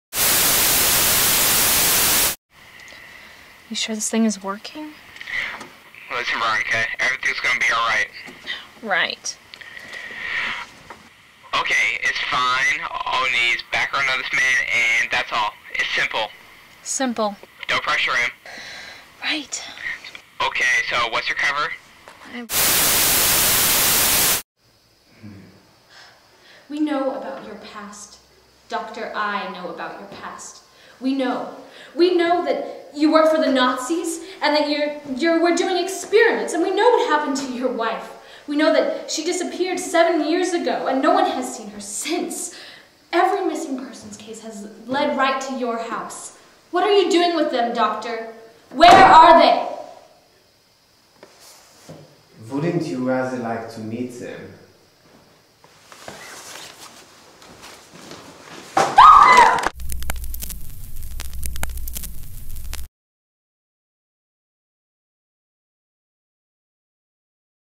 Dr. Helldorf's Manor Short Trailer
The shortened trailer to Dr. Helldorf's Haunted Manor. This Halloween, Revolution Youth is presenting a Halloween event at Kitsap Sports in Silverdale. The Halloween event consists of a haunted mansion and many of activities including carnival games. Admission is free, so don't miss out on this event!